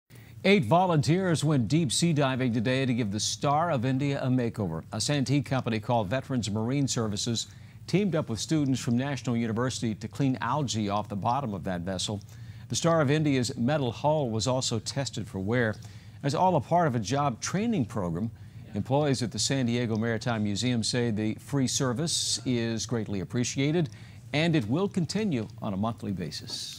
Volunteer divers performing underwater cleaning
KUSI clip about volunteer divers cleaning our ships, all part of a job training program.